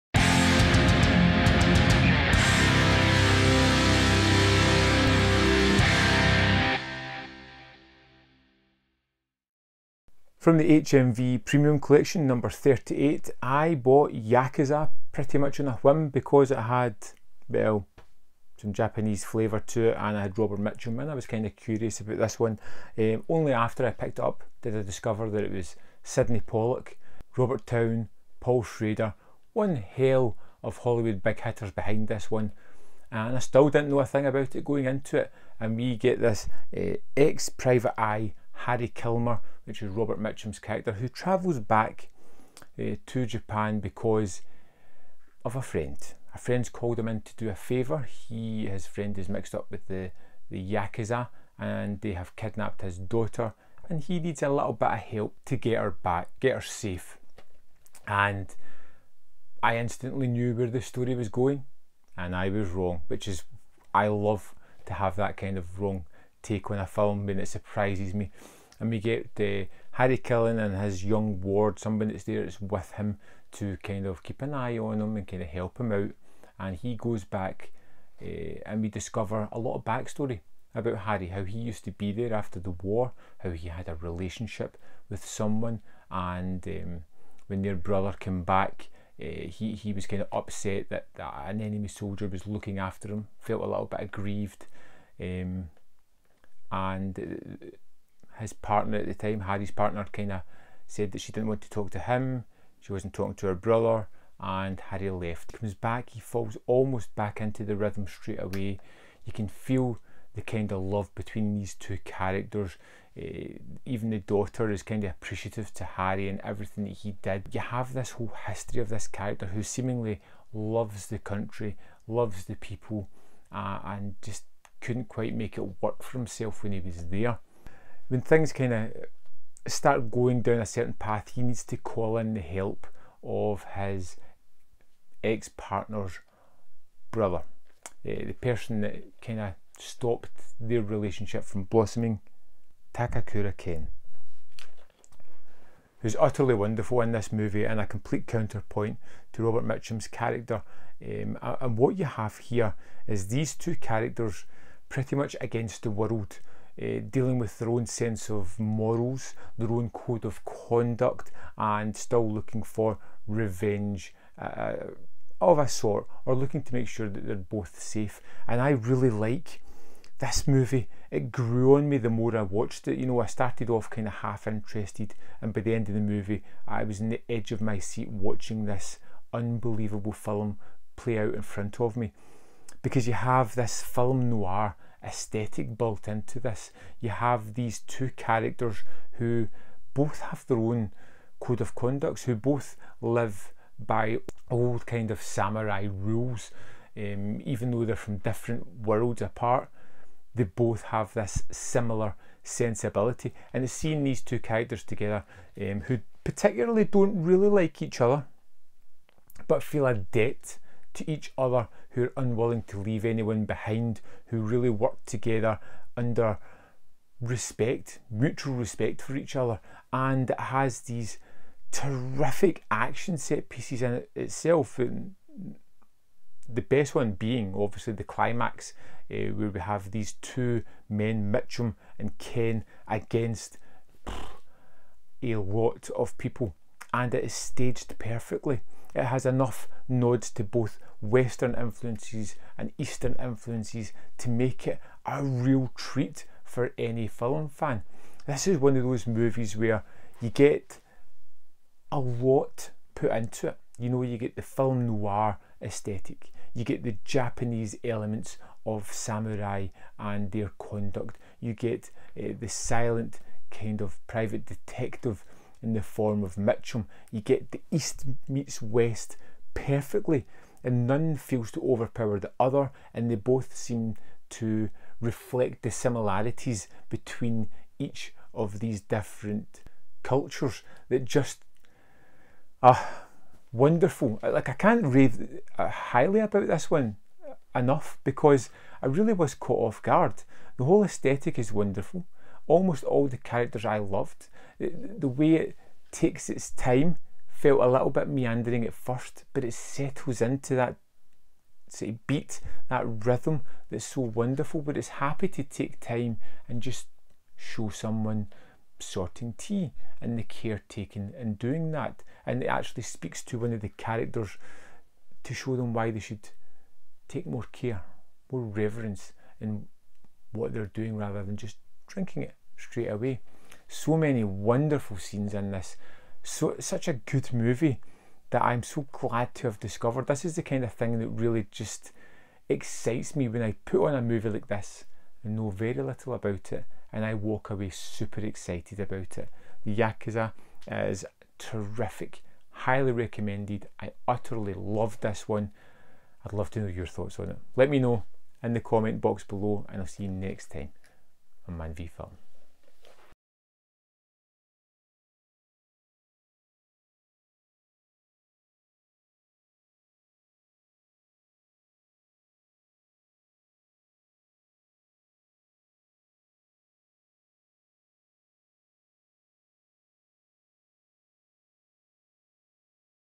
Yakuza | 1974 | Movie Review | HMV Premium Collection #38 | Sydney Pollack
American private-eye Harry Kilmer returns to Japan to rescue a friend's kidnapped daughter from the clutches of the Yakuza.

Movie Review for The Yakuza